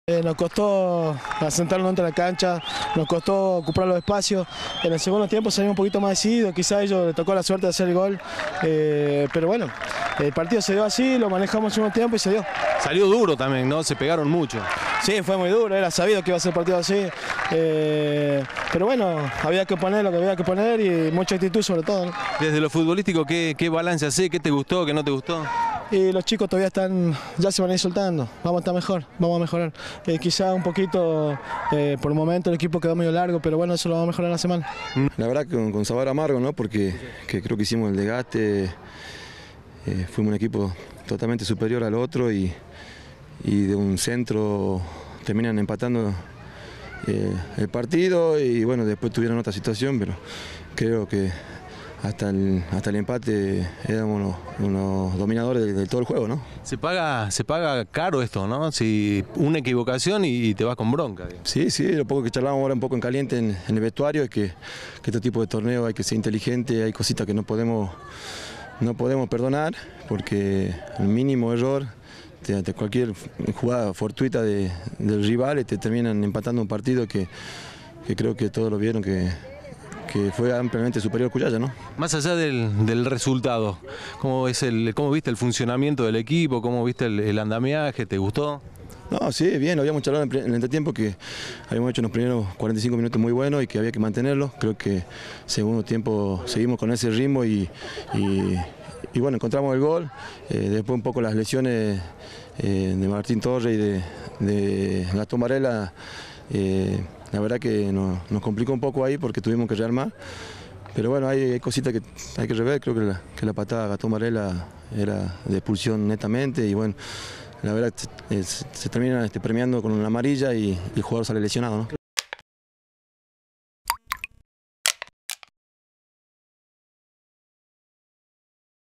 Cuyaya y La Viña empataron 1 a 1 en el inicio del Torneo Regional Federal Amateur
Los representantes capitalinos cumplieron con el primer compromiso de la temporada.